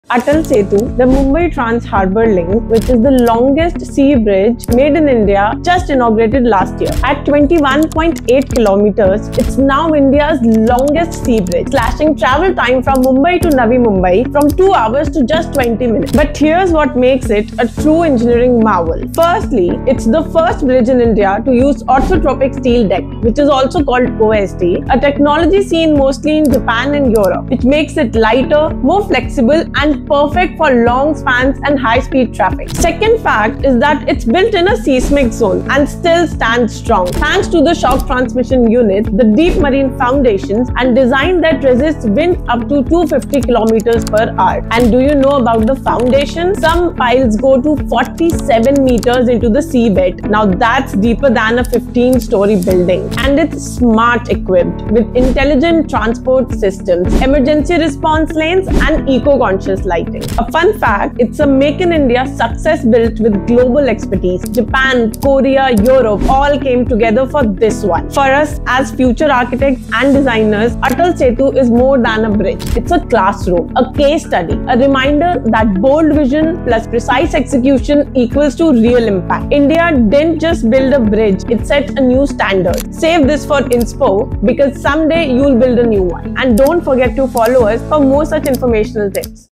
Atal Setu-Indian's Longest Sea Bridge
Atal Setu (MTHL) is now open — India’s longest sea bridge is here to rewrite commute history! 🌉🇮🇳

Stretching 21.8 km (with 16.5 km over the sea), this engineering marvel is set to handle over 70,000 vehicles daily, slashing the Mumbai to Navi Mumbai travel time to just 20 minutes — down from a 42-km drive. 🚗⚡

More than just a bridge, it’s a symbol of India’s infrastructure leap — powered by the hard work of 17,000+ laborers and 1,500 engineers, creating massive employment in the process. 👷‍♂️🏗️📈

A proud step towards a developed Bharat. 💪🌍